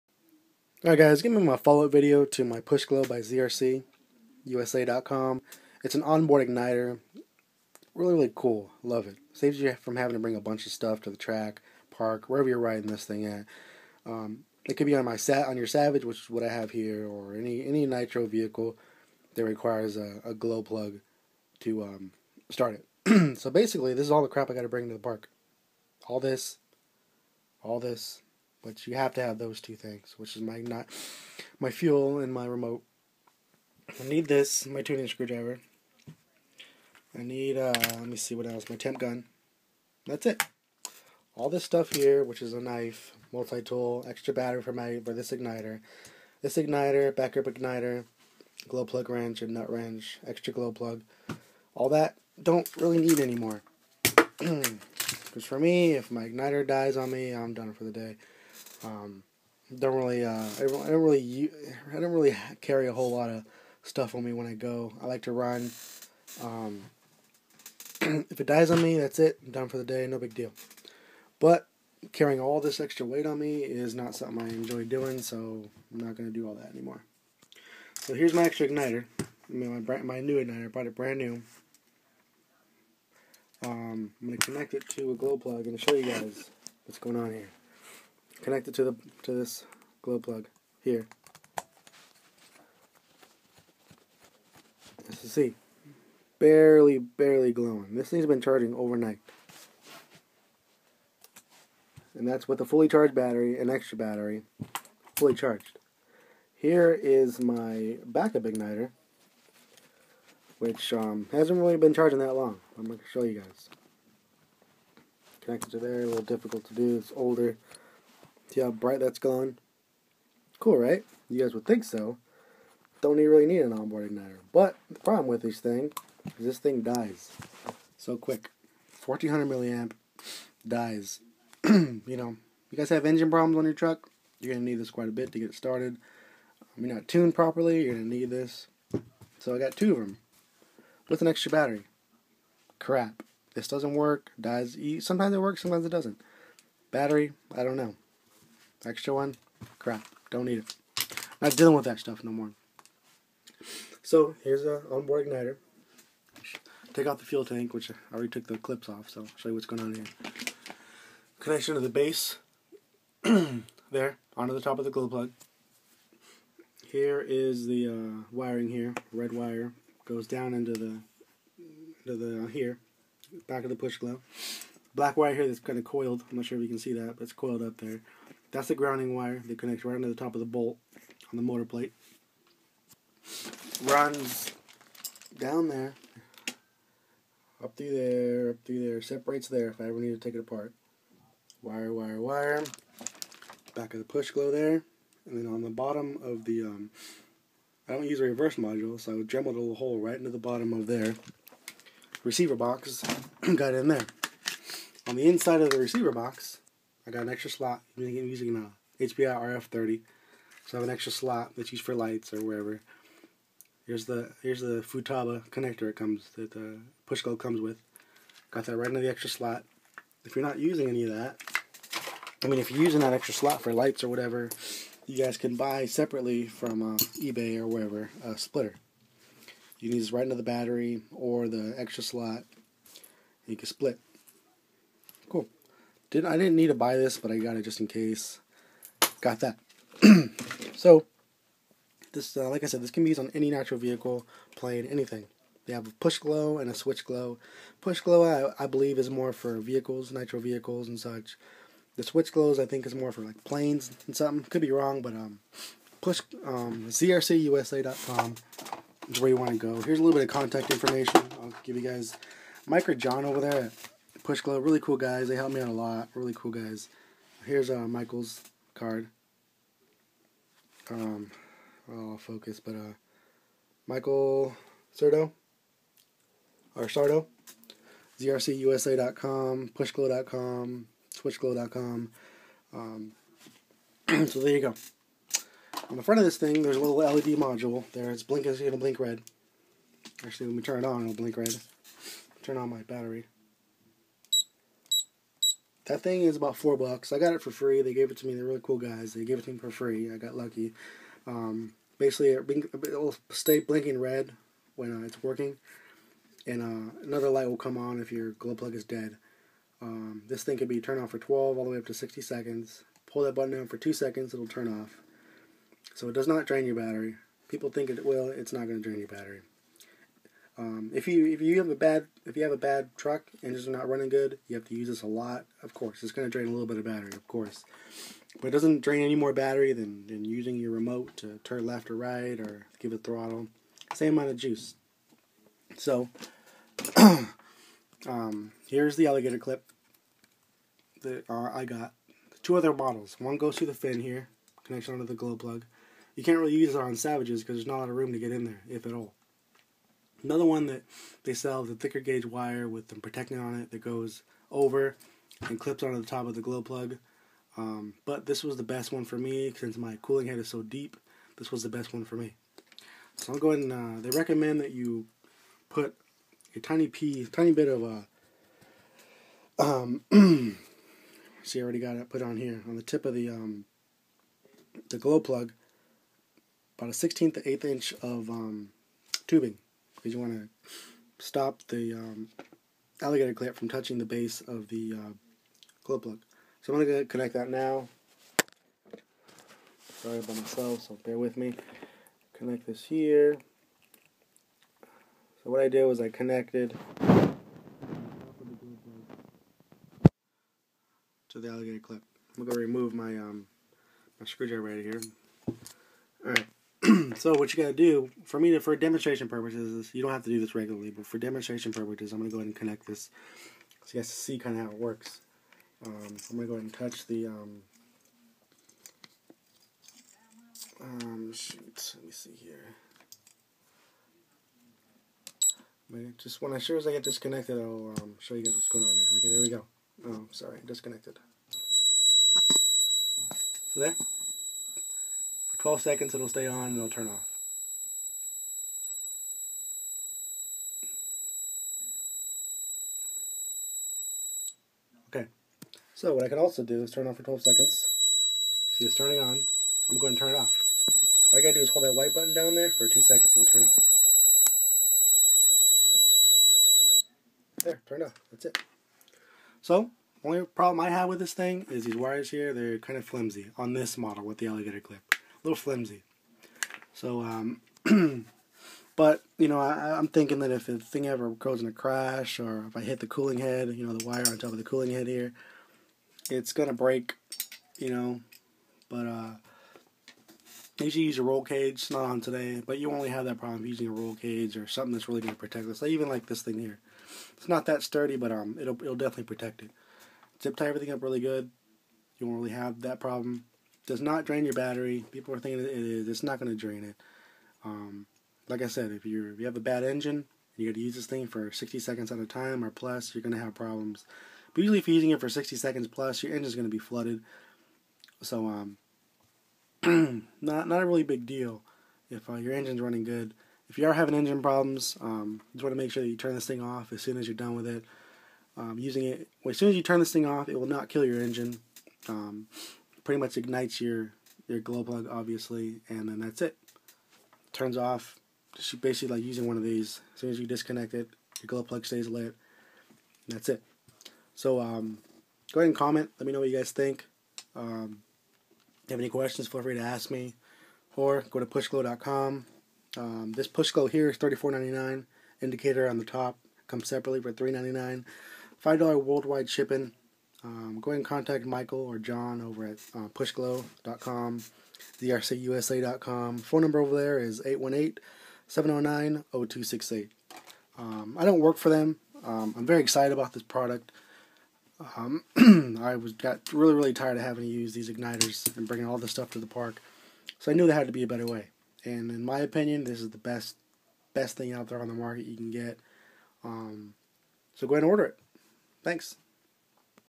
Pushglo by ZRC Review Part 2 (Updated)
A more in depth review of my onboard igniter by ZRC